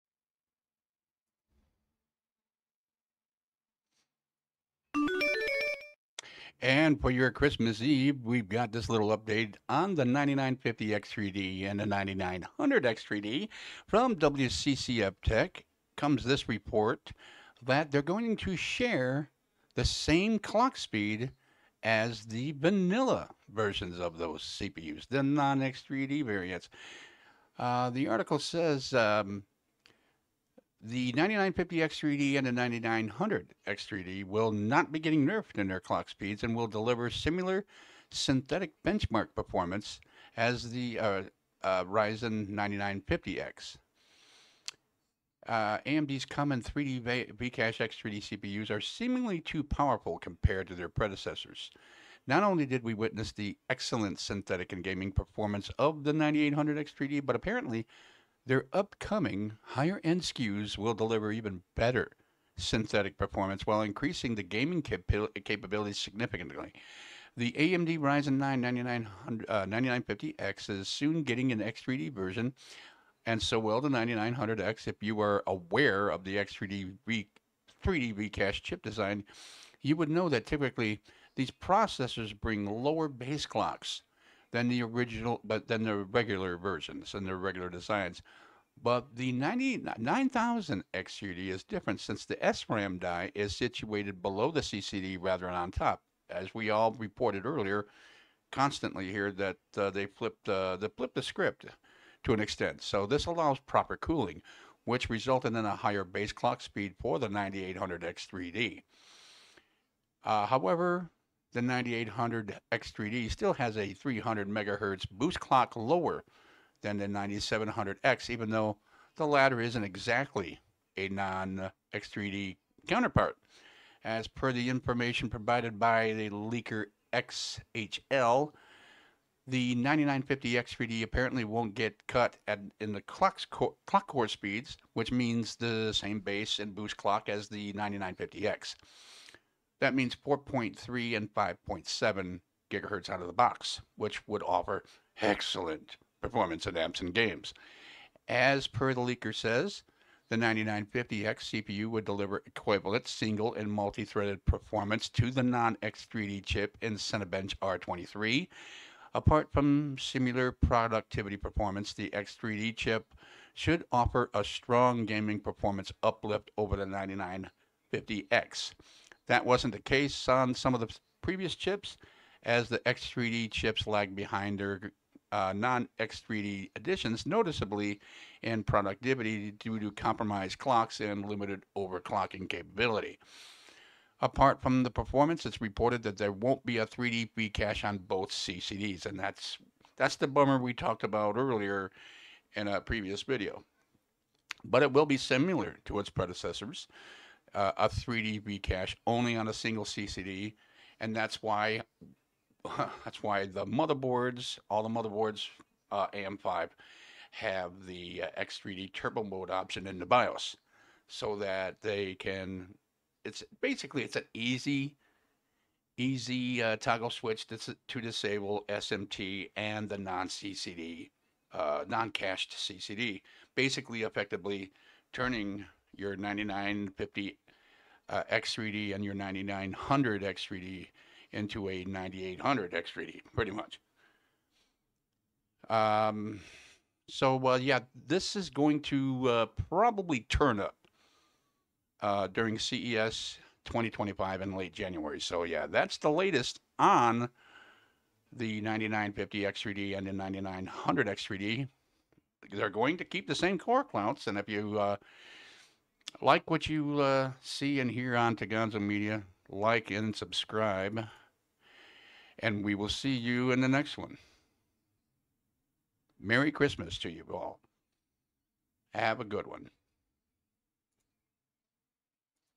9900X3D & 9950X3D Update
The 9900X3D & 9950X3D will retain same clocks as Non-X3D variants ...